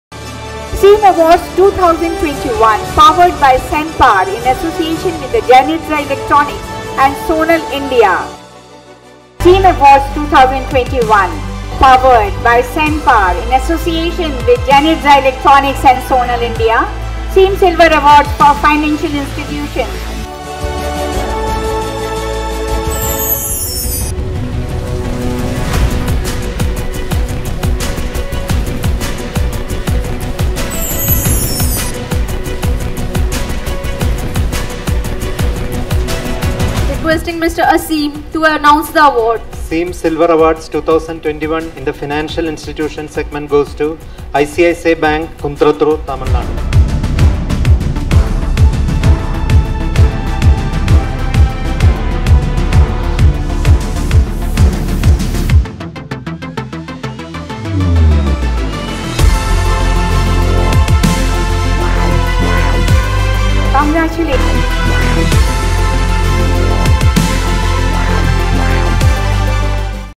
SEEM SILVER AWARD 2021 : ICICI Bank Ltd, Kundrathur
SEEM SILVER AWARD for Financial Institutions : ICICI Bank Ltd, Kundrathur, Tamil Nadu
SEEM AWARDS 2021  |  Powered by SANPAR  |  In Association with JANITZA & SONEL held at New Delhi on September 2022.